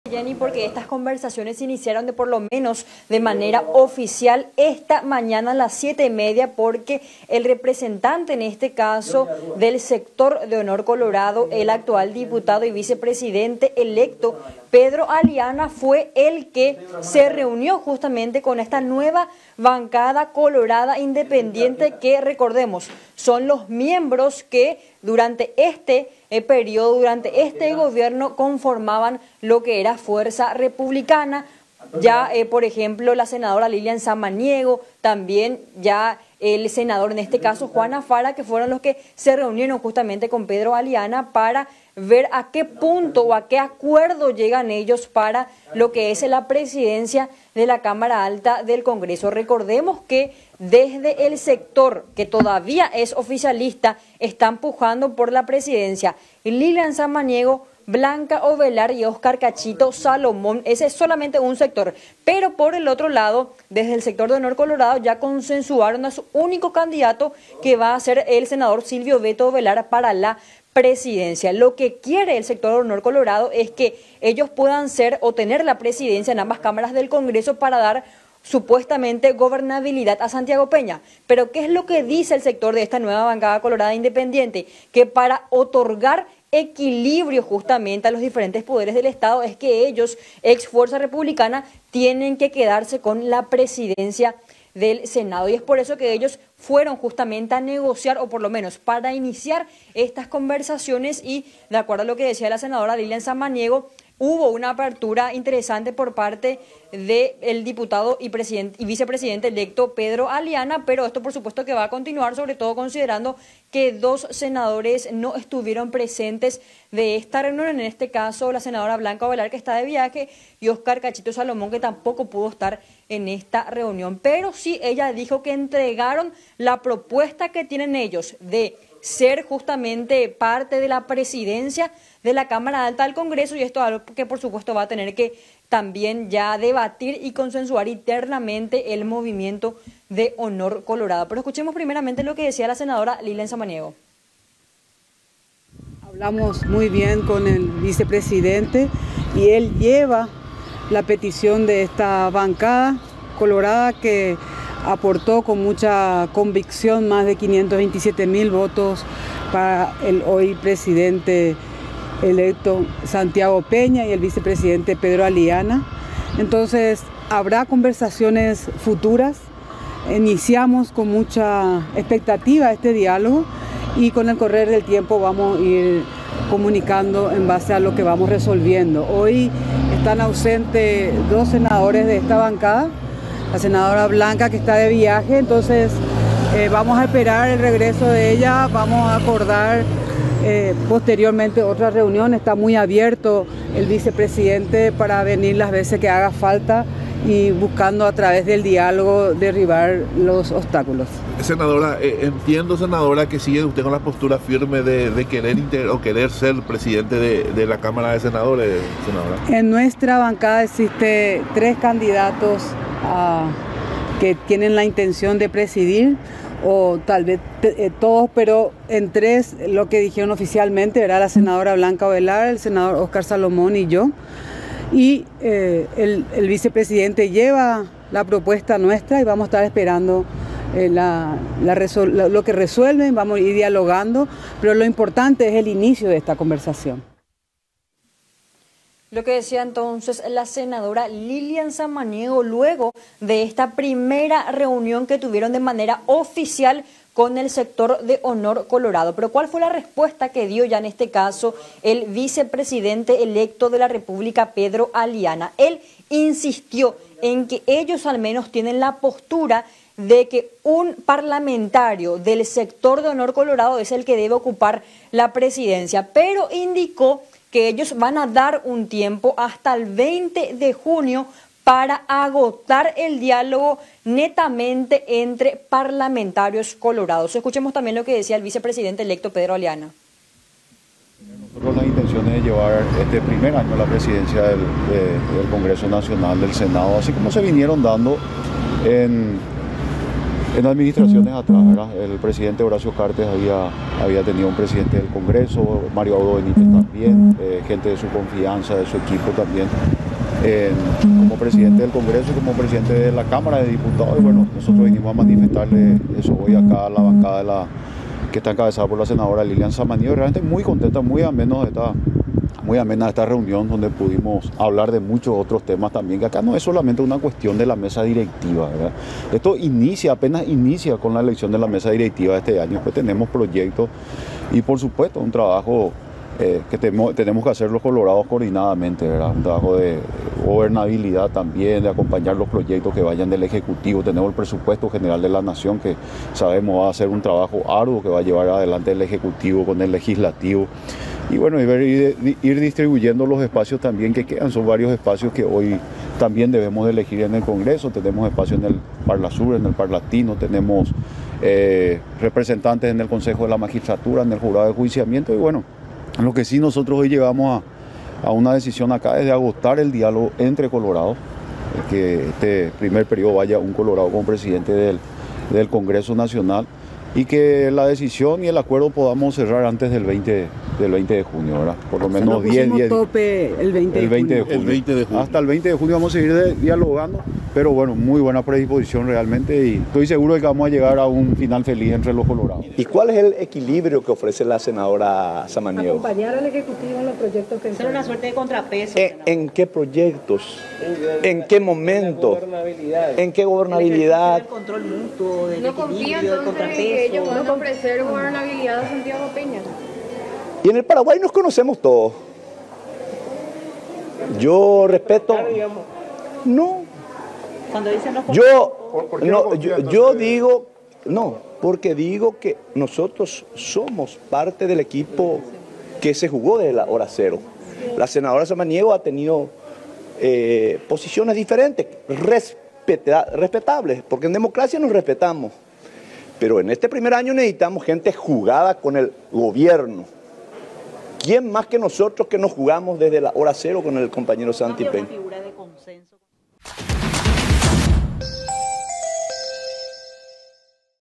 Continúa puja por presidencia del Senado
En el Senado continúa la puja entre los parlamentarios colorados para copar la presidencia de esta Cámara del Congreso. Esta mañana se realizó la primera reunión entre la bancada de Honor Colorado y la nueva bancada “independiente” y, si bien aún no llegaron a un acuerdo, los legisladores expresaron que tienen la intención de agotar el diálogo entre correligionarios para este 20 de junio.